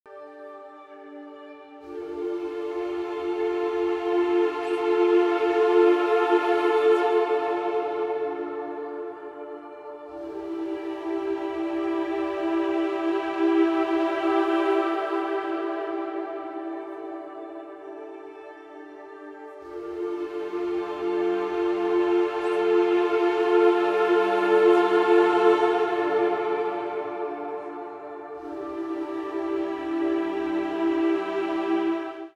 Once in 80,000 years: Rare comet crosses sky | USA TODAY
Stargazers in California saw the newly-discovered comet C/2023 A3 lighting up the sky.